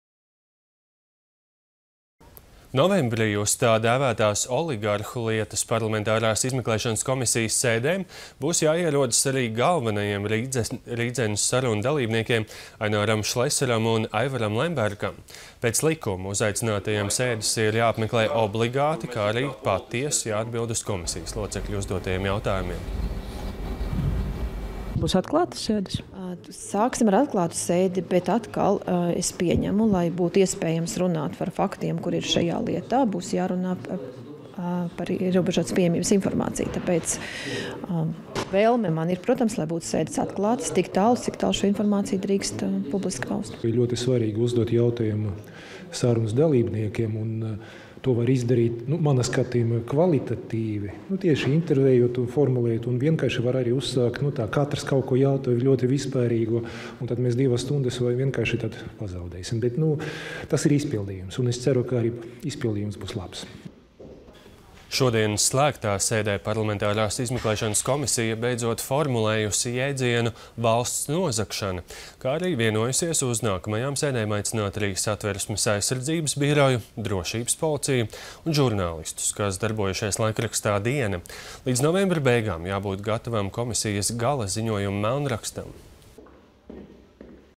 Uz Saeimu aicinās Šleseru un Lembergu
Novembrī uz tā dēvētās oligarhu lietas parlamentārās izmeklēšanas komisijas sēdēm būs jāierodas arī galvenajiem “Rīdzenes” sarunu dalībniekiem Aināram Šleseram un Aivaram Lembergam.